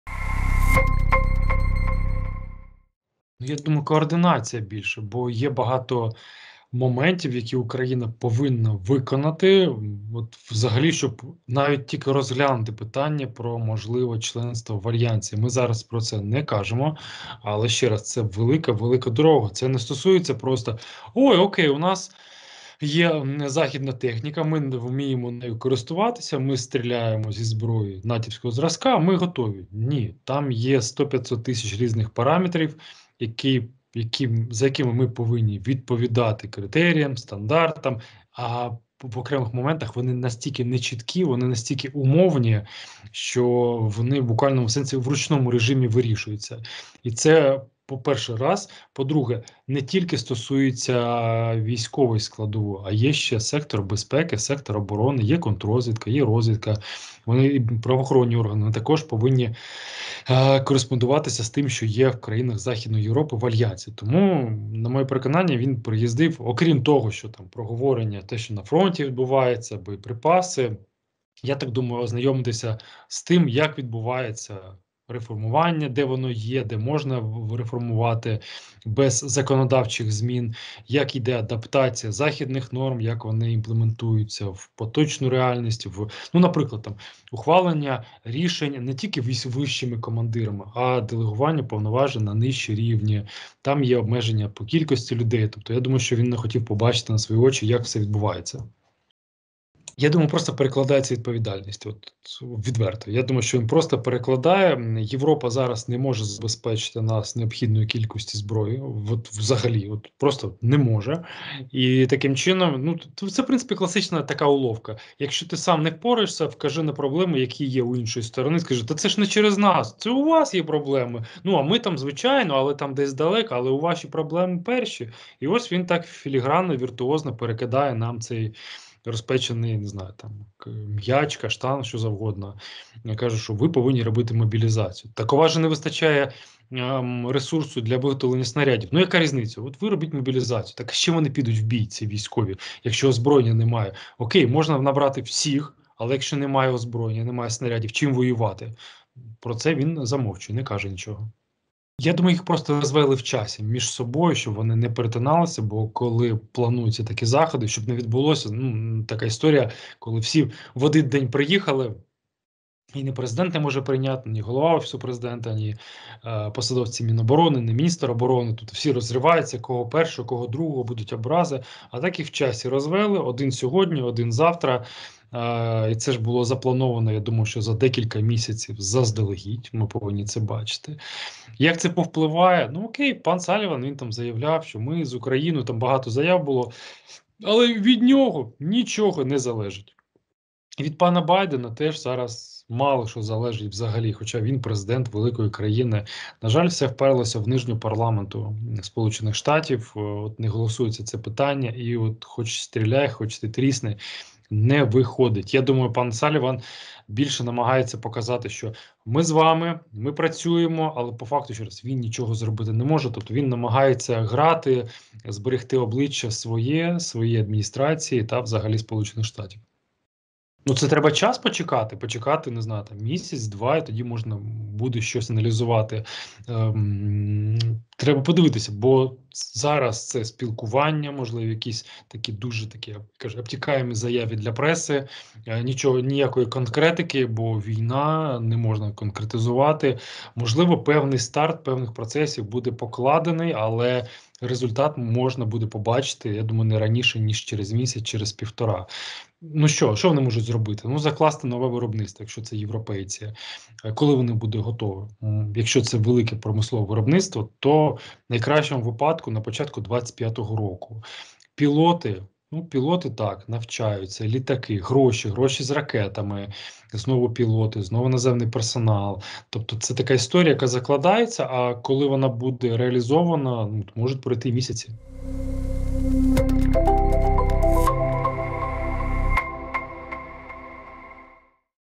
Київ відвідала перша військова делегація НАТО на чолі з Робом Бауером
Під час виступу на Київському безпековому форумі очільник Військового комітету НАТО Роб Бауер заявив, що для подальшого ведення війни Україна потребує не лише зброї, а й людей. Тому їй  необхідно провести додаткову мобілізацію, щоб замінити загиблих і поранених. Офіцер також закликав суспільство вірити у перемогу України та наголосив на подальшій підтримці Києва. Про мету цього візиту, а також про те, чи пов'язаний він з приїздом до Києва радника президента США з нацбезпеки Джейка Саллівана, розповідає військовий експерт Іван Ступак.